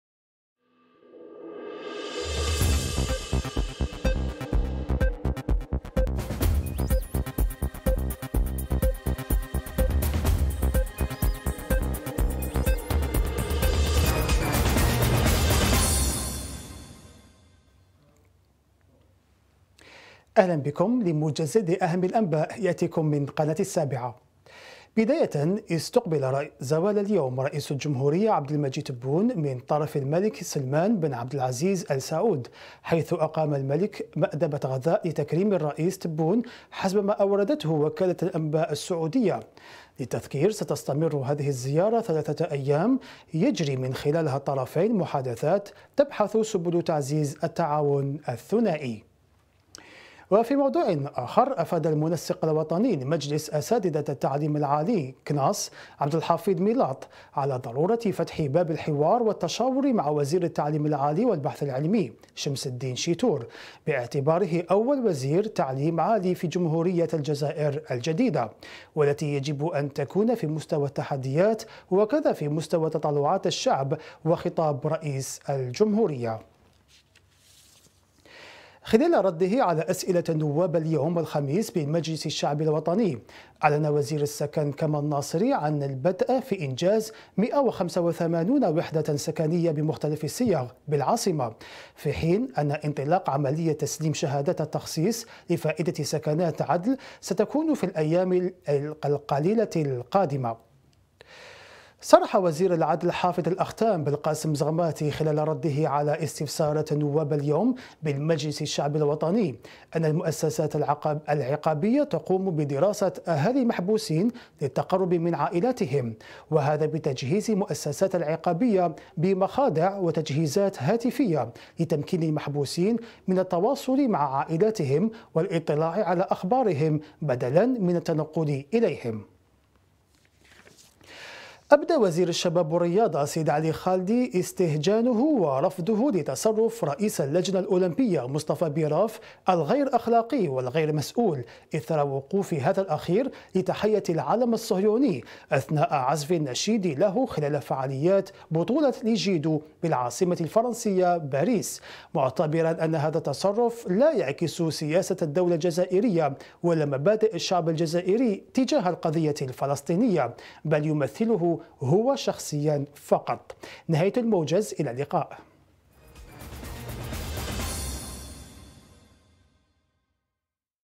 🛑 الموجز الإخباري
🛑 العاهل الملك سلمان يستقبل الرئيس تبون
🛑 نقابة الأساتدة التعليم العالي تفتح حوار مع وزير التعليم العالي
🛑 إنجاز 185 ألف وحدة سكنية بمختلف الصيغ قريبا
🛑 إستحداث خدمة جديدة للمحبوسين
🛑 وزير الشباب و الرياضة يفتح النار على بيراف
-2:40